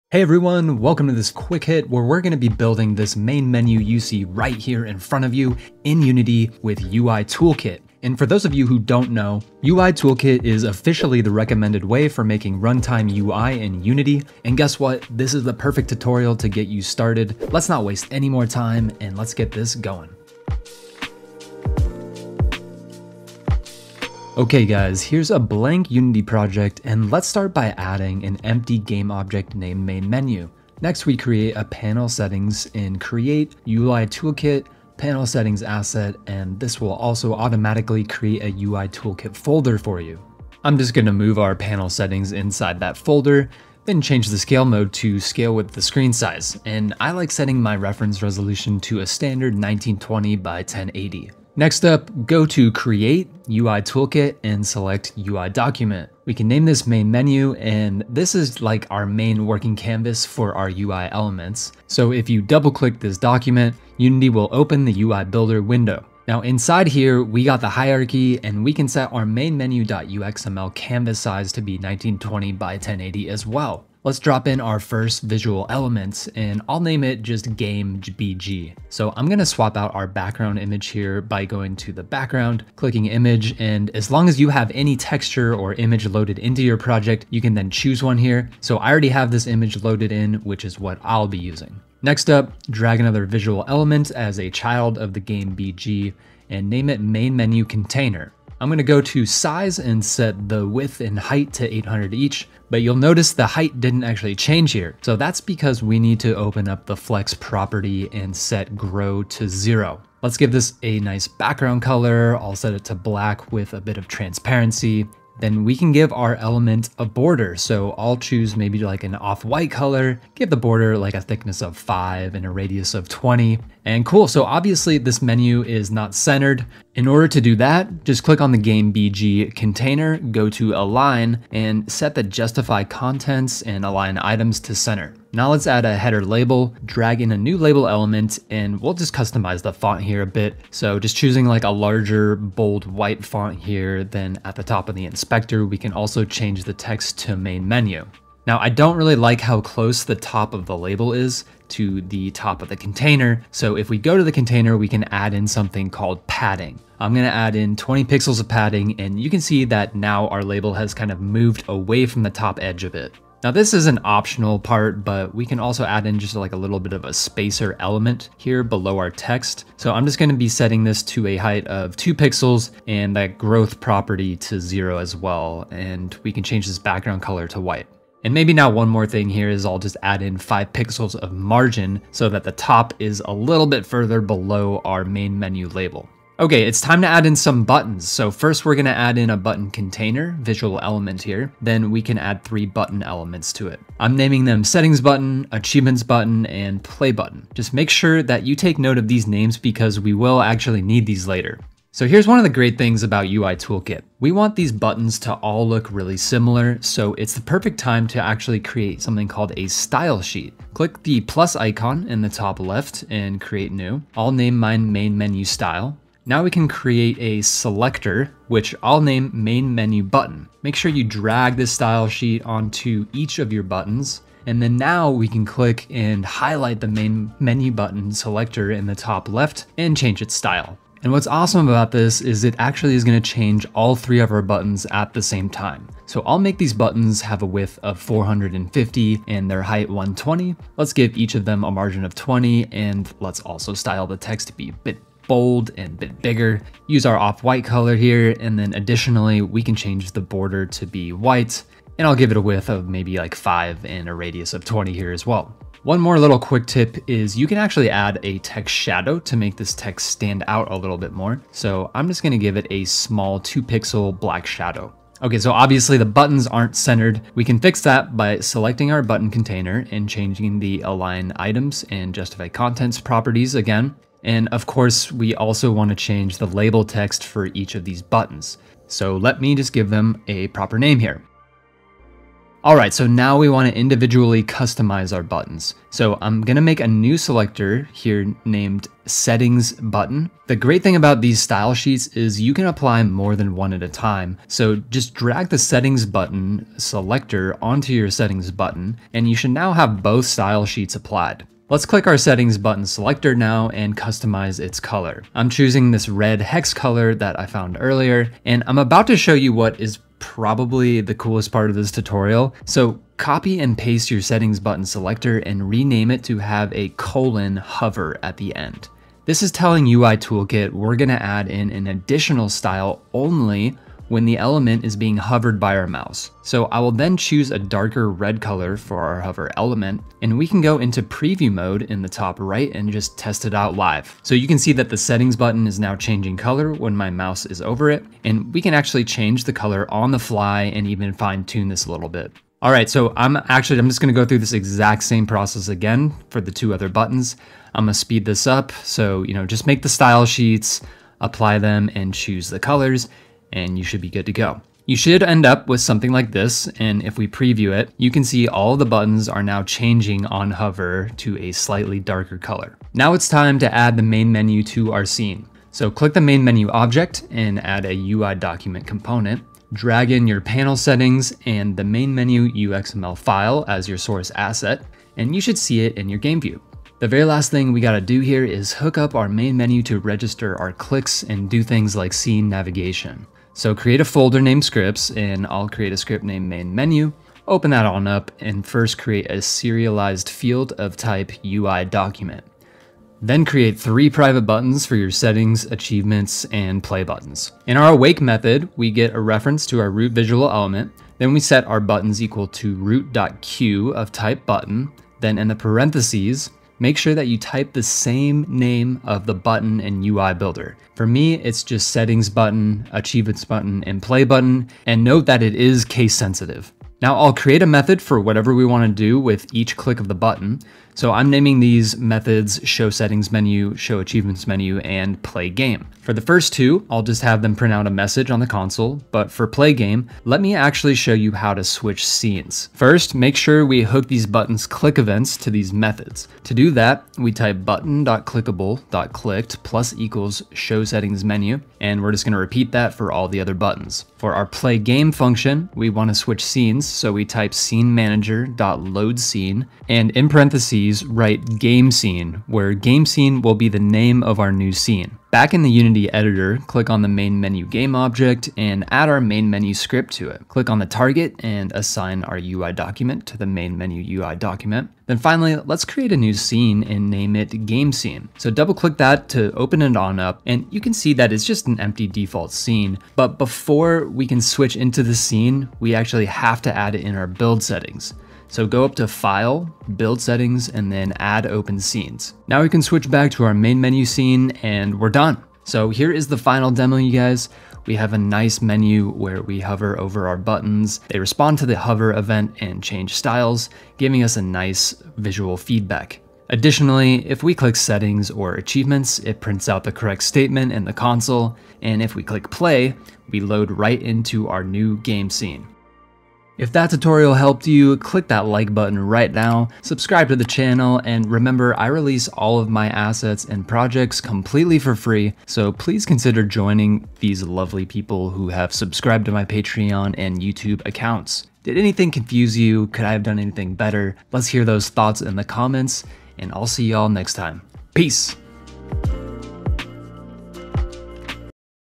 The slick new way to make Main Menus
In this tutorial we go through the basics of making a main menu in UI Toolkit. This uses UI Builder, which is similar to a web dev approach to UI in Unity. We learn how to use hover events, clickable buttons, and navigating between scenes so that you can create a main menu scene and a game scene and switch between them.

Chapters

0:00 Intro
0:23 Main Menu Container
3:05 Adding Buttons
4:42 Styling While Hovering
6:17 Click Events in C#
8:04 Demo